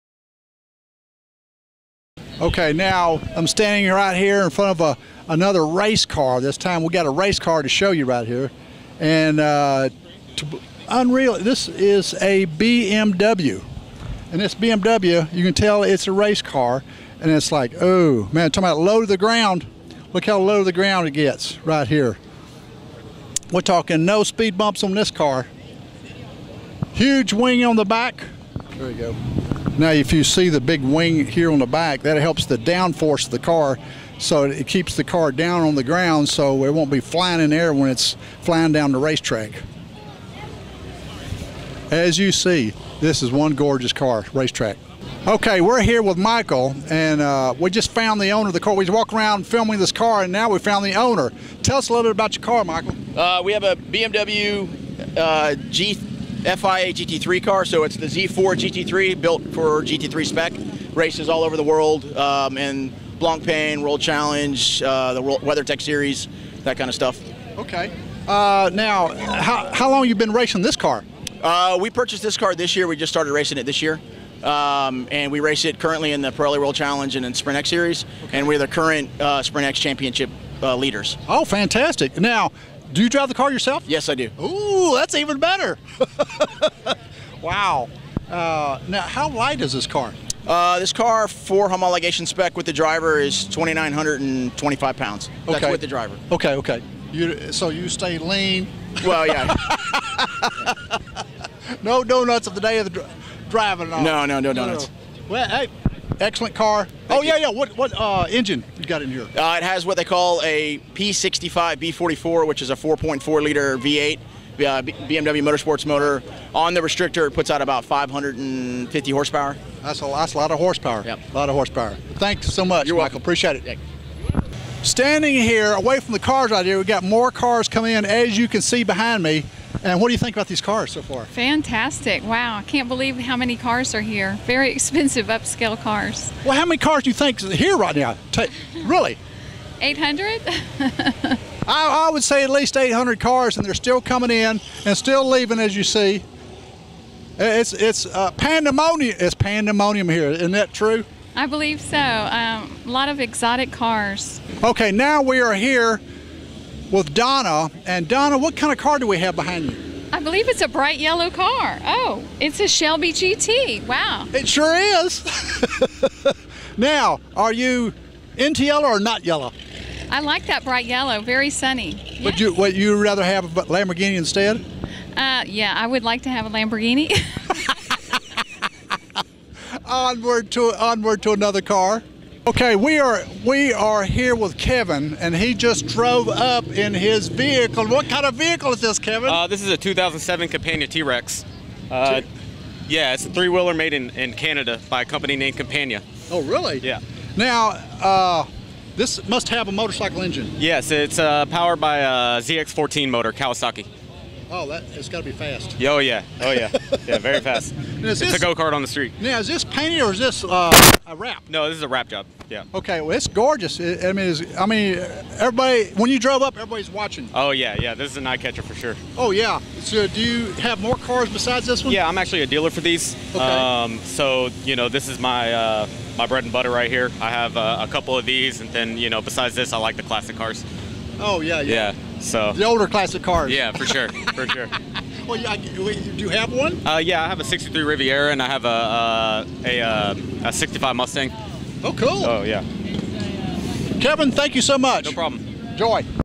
Coffee & Cars Event (3) in Houston
"G" is interviewing more owners of the Coffee & Cars event. Donna and "G"  you will see talking about the BMW race car with a Shelby GT and last a T-Rex 2nd video to be shown.